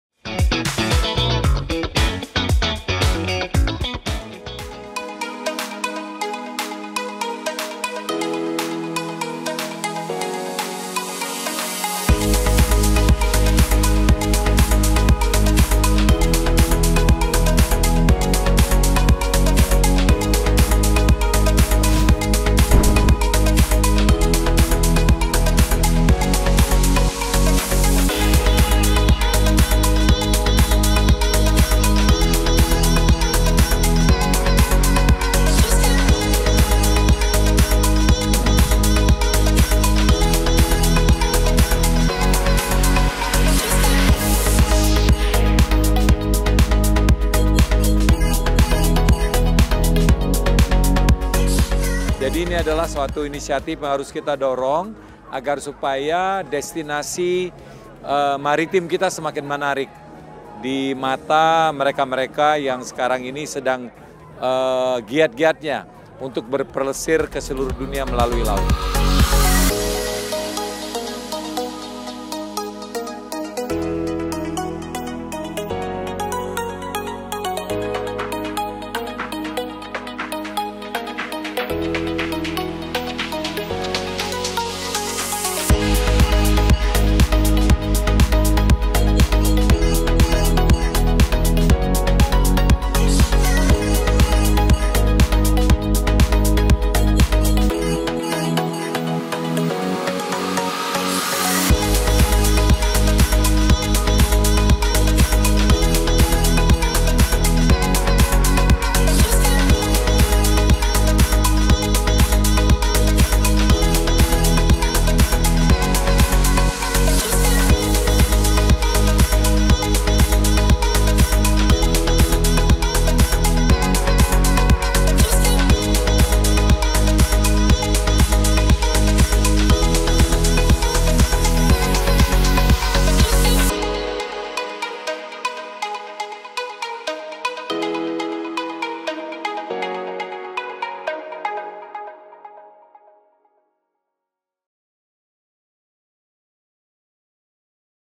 Sail to Indonesia & Benoa Port Jazz Festival Goes to BMTH (After Movie Day 3)
Cuplikan keseruan hari ke-3 Sail to Indonesia Goes to BMTH!🛥️❤️

Terimakasih kepada semua yang sudah datang dan meramaikan Sail to Indonesia Goes to BMTH hari ke-3!🙌